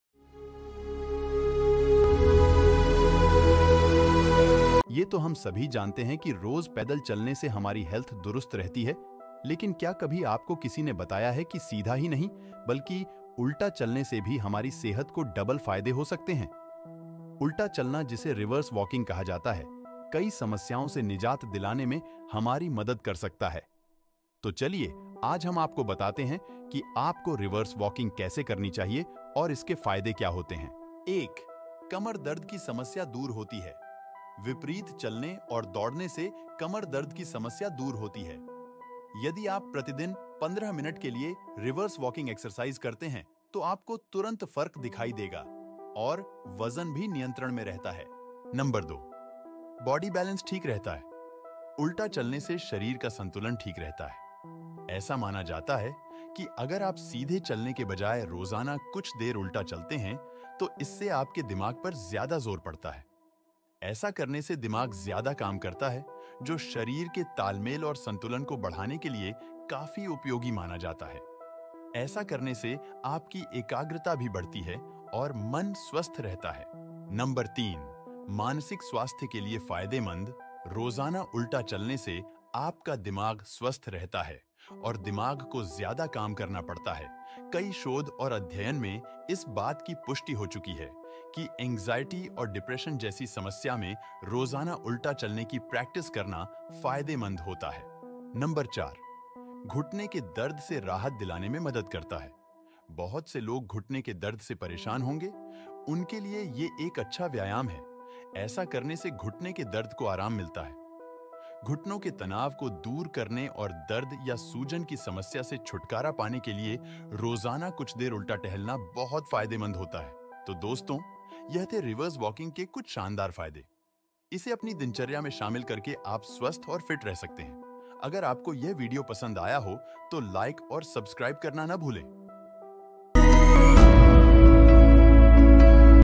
Reverse Walking के जबरदस्त फायदे || Healthy Talks with Shafiq Khan
Ever feel like you're stuck in a rut, both literally and figuratively? Then put on your shoes and get ready to walk backwards! That's right, this seemingly silly activity unlocks a surprising world of benefits for your body and brain.

In this video, we'll dive into the fascinating world of reverse walking:

Boost your fitness: Engage different muscle groups, burn more calories, and improve balance and coordination.
Challenge your brain: Step outside your comfort zone and stimulate new neural pathways.
Improve knee health: Gently strengthen and protect your knees with this low-impact movement.
Unlock hidden benefits: Discover how reverse walking can aid in foot health, downhill control, and even running performance!
Get started safely: Learn proper technique, safety tips, and progressions for all levels. ‍
Whether you're a seasoned athlete or a fitness newbie, reverse walking offers something for everyone. So join us on this journey and discover the joy of moving in reverse!